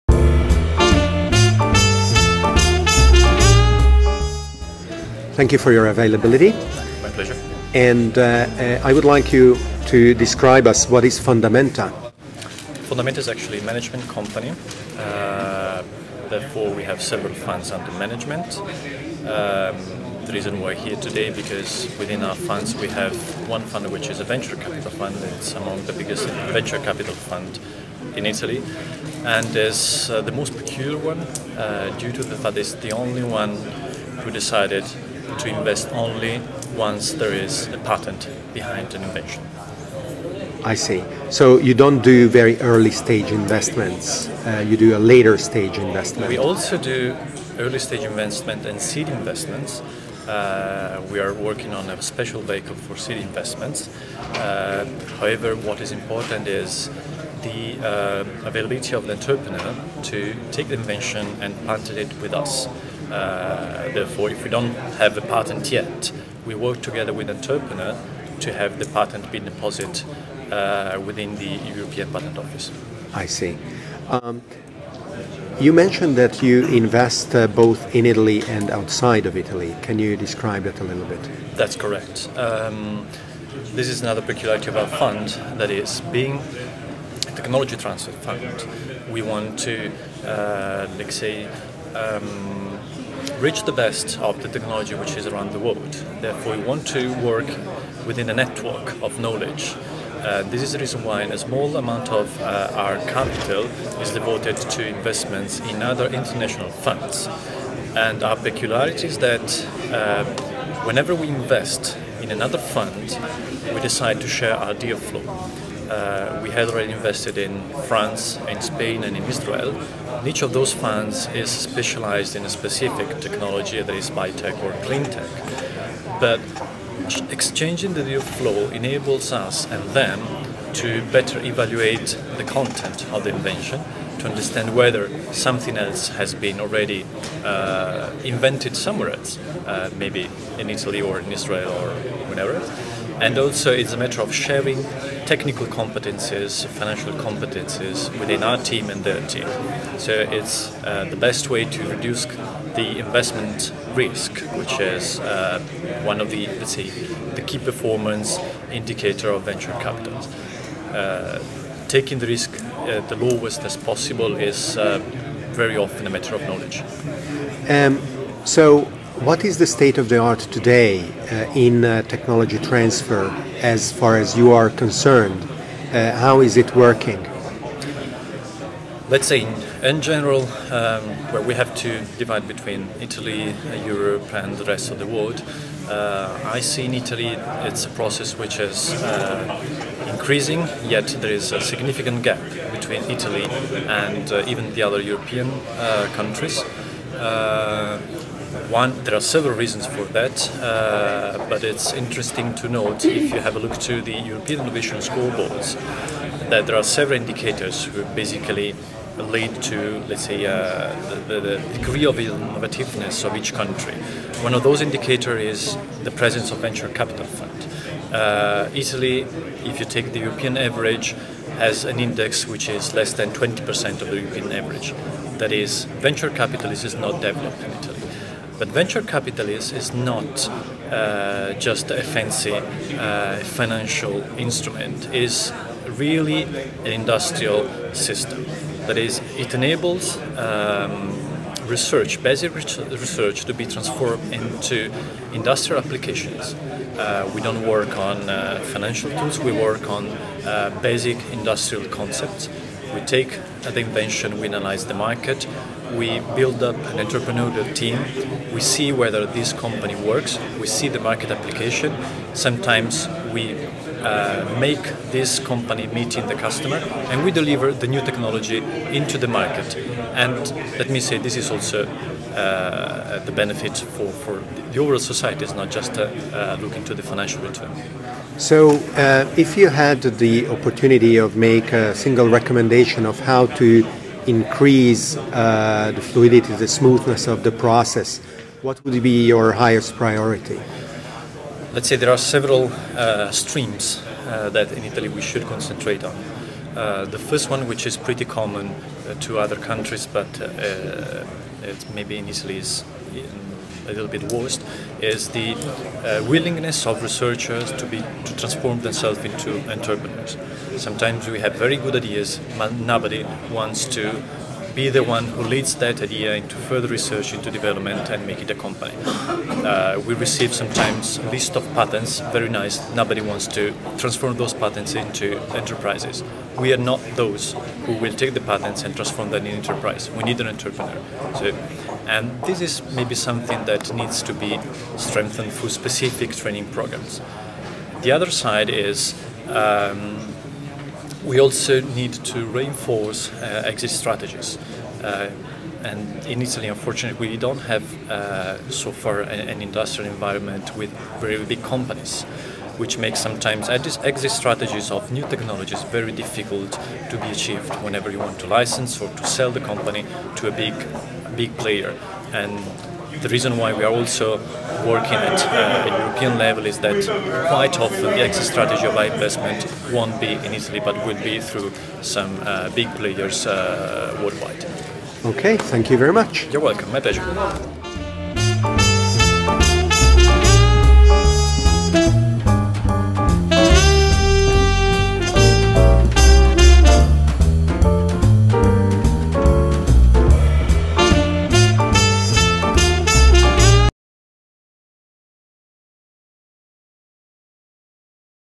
TechTransfer 8 - Fondamenta SGR, TT Venture
How does technology transfer work between public research entities, universities, and industry? Interviews at a conference organized on the subject by LES.